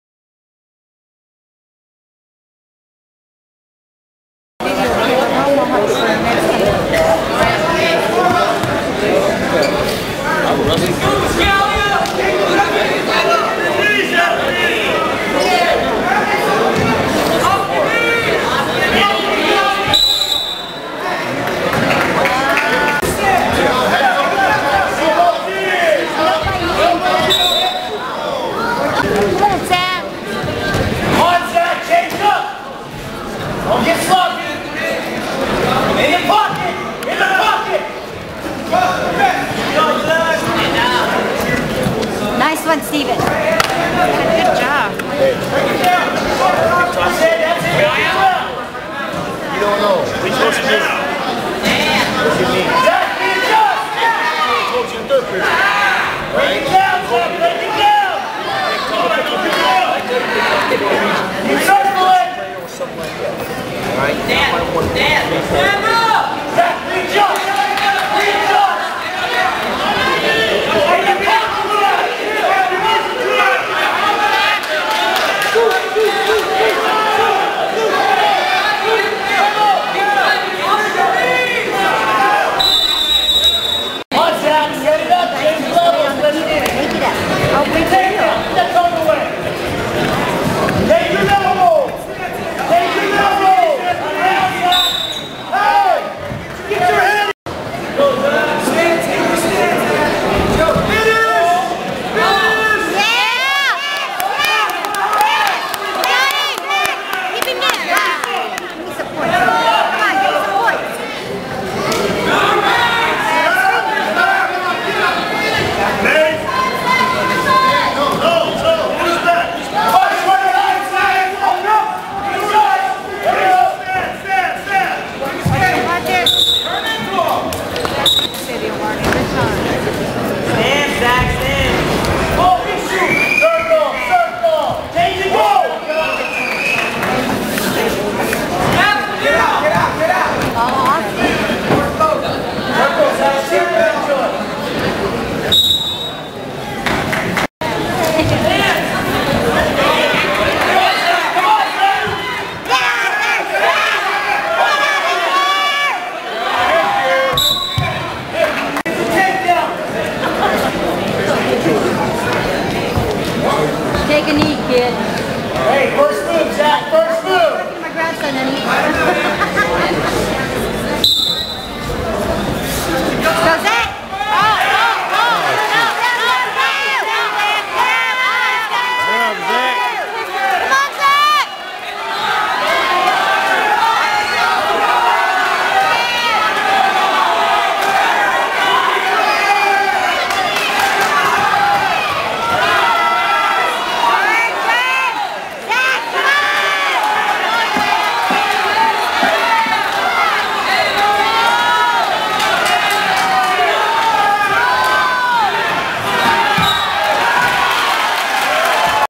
Elness vs. Buscaglia.wmv
Zach Elness 140 lbs. vs. Anthony Bascaglia of St. Mary's preseson dual 12-3-08